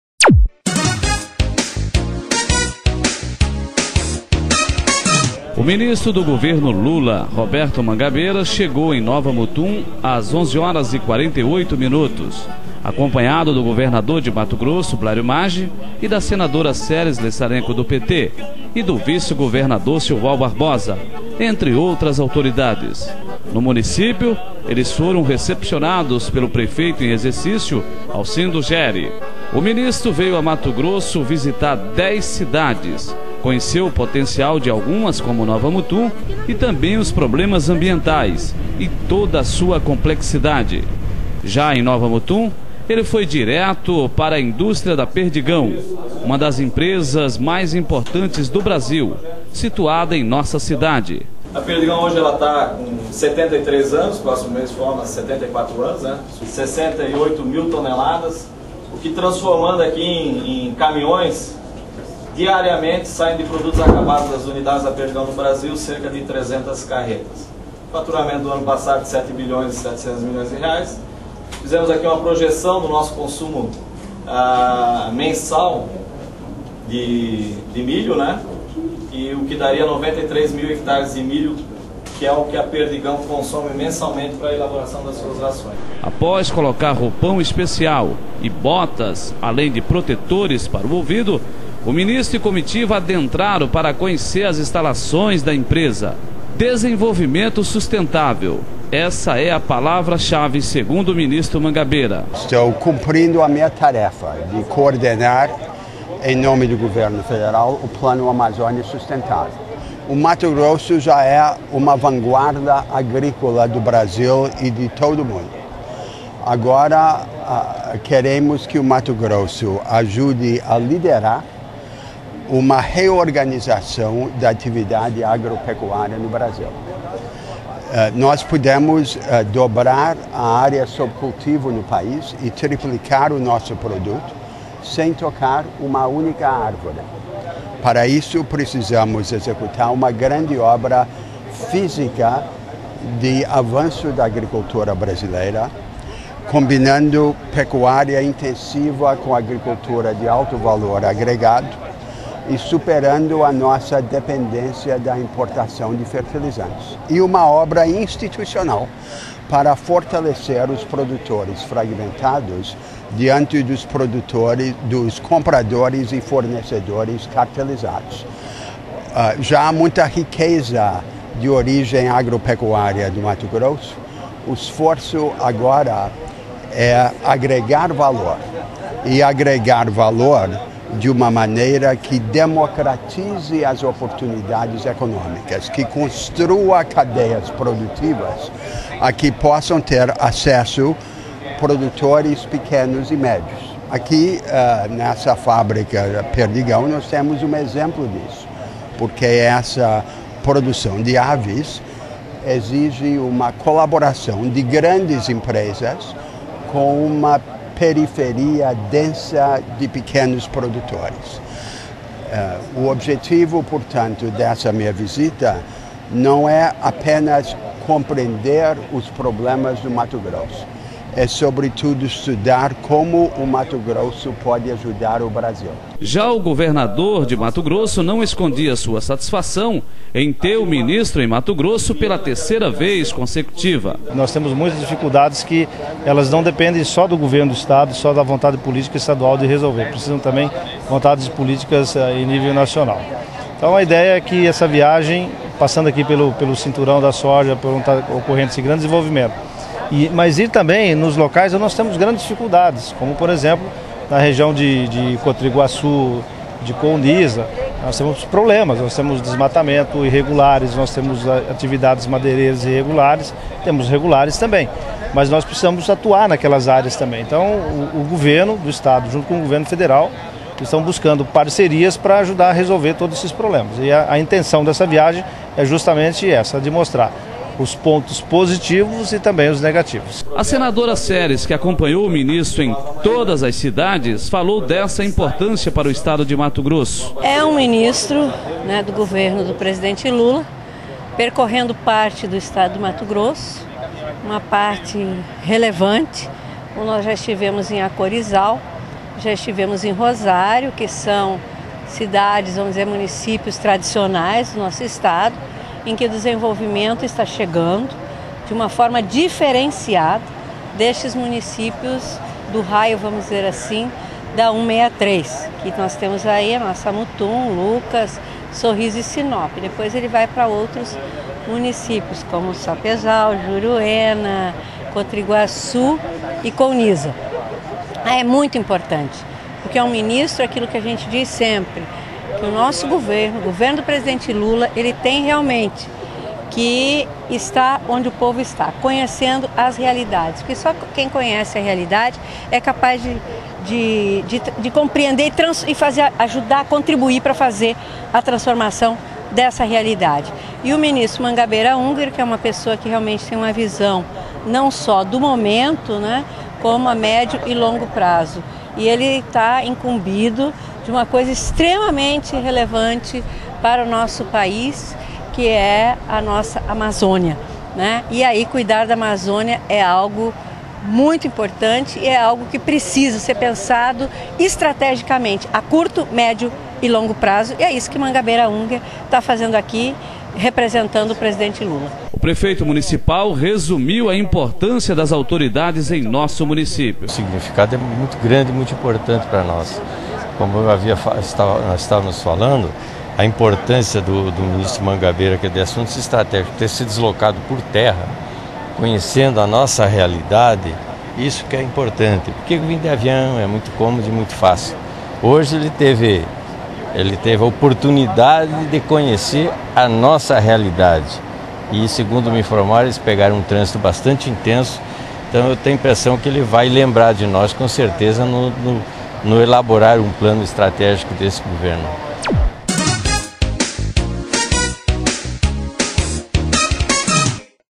Ministro Manguabeira Unger em visita a Nova Mutum
Ministro Manguabeira Unger em visita a Nova Mutum conheçe a Unidade Pergidão Agroindustrial S/A nova Mutum em toda a sua Produção diária de 200 mil aves de abate dia.